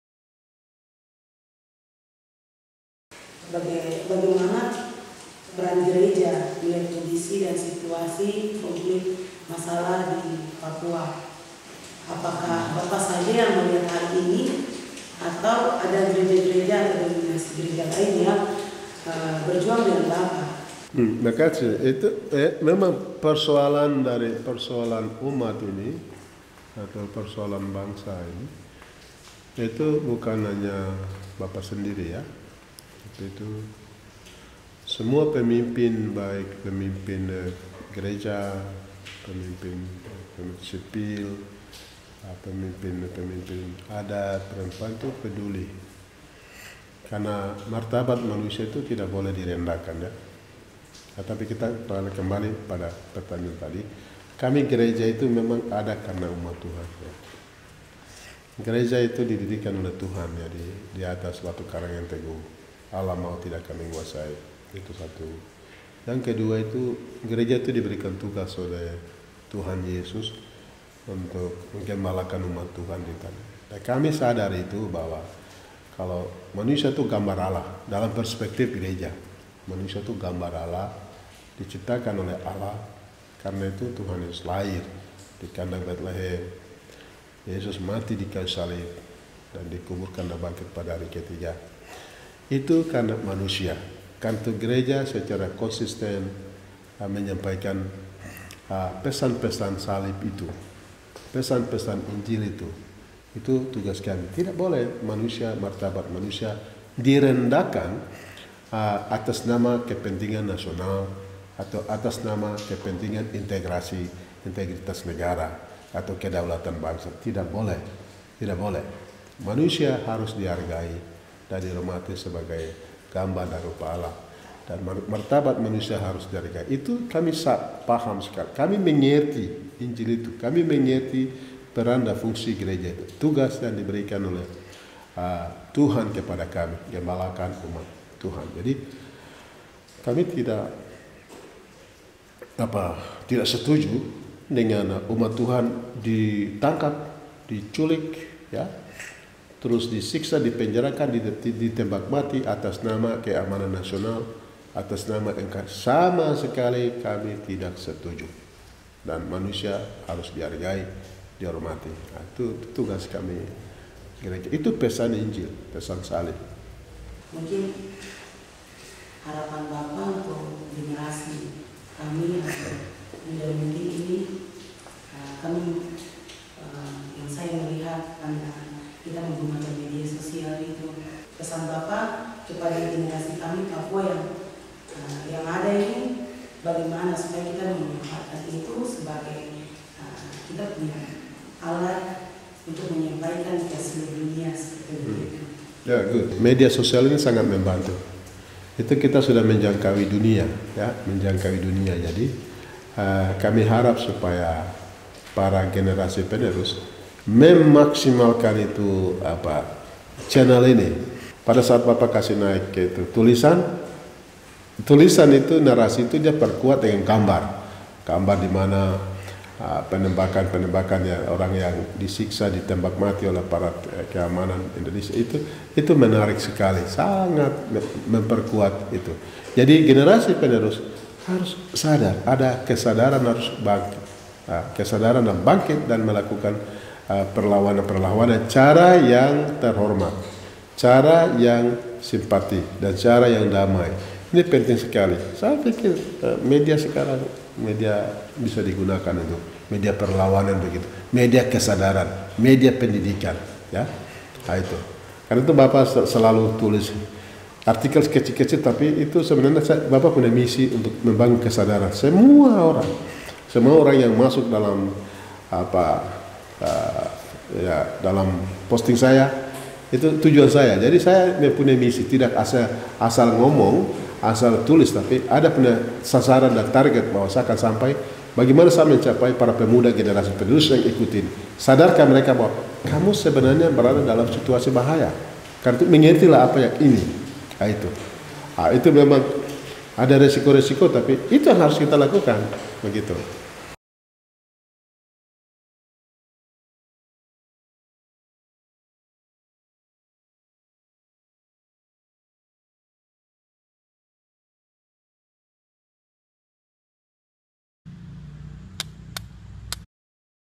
Wawancara Dr Socratez Yoman oleh Oder Pawika
Oder Pawika, juru kamera dan pembuat film muda asli Papua mewawancara Gembala Pdt Dr Socratez Yoman di kantornya.
Produksi Kelompok Anggrek Hitam, 2018